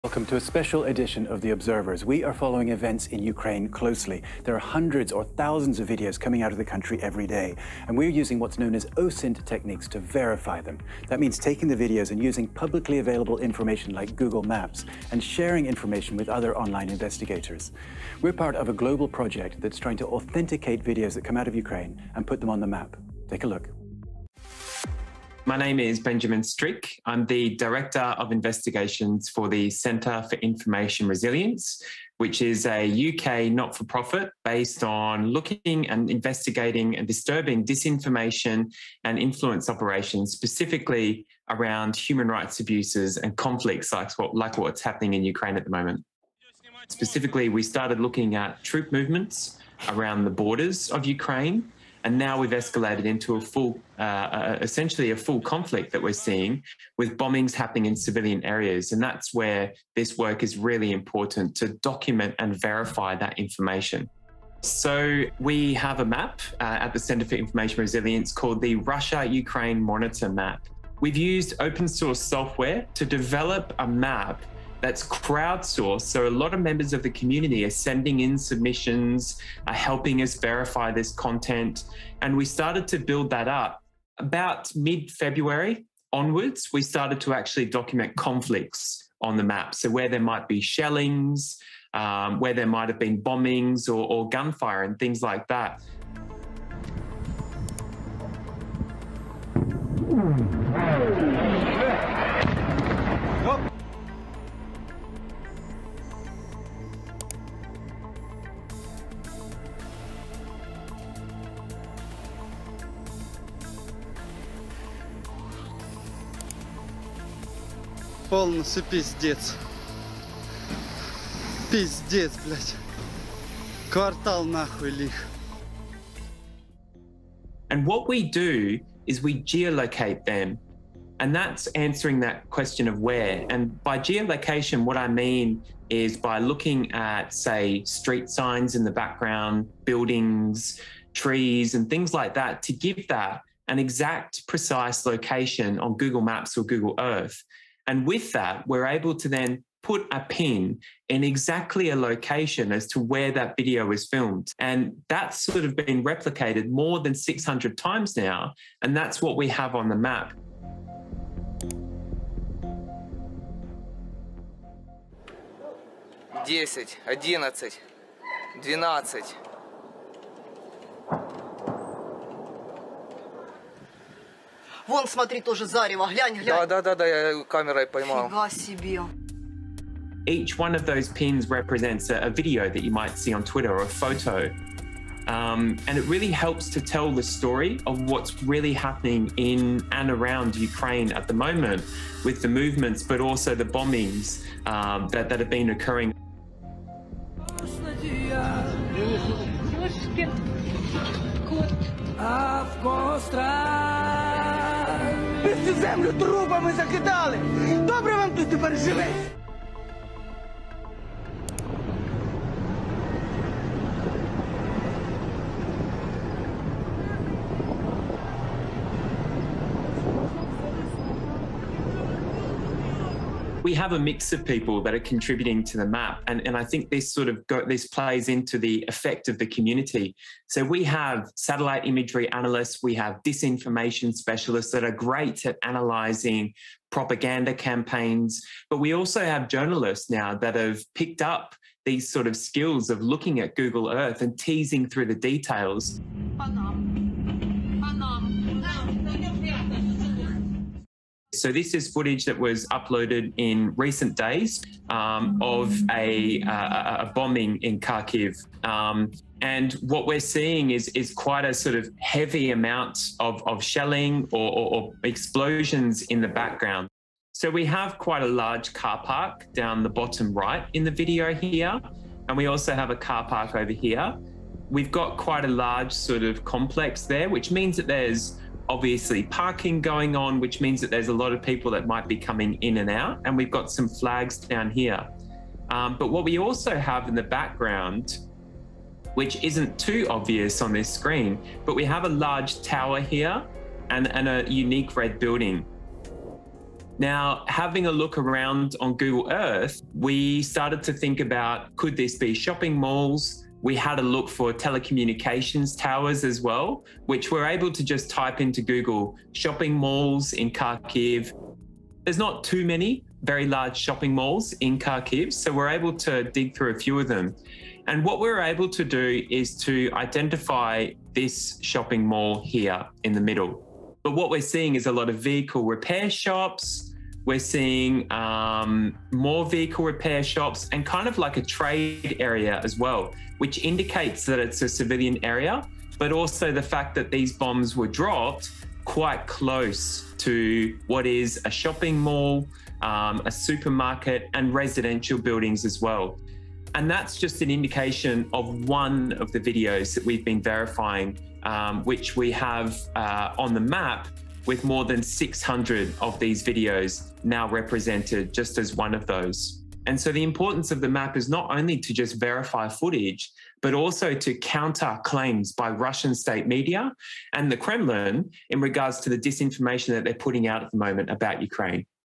Online investigators are compiling and verifying videos of the war in Ukraine with this map
The Observers program brings you the best amateur images from around the world, all verified by France 24 journalists. To see more reports from our Observers – and to become an Observer yourself!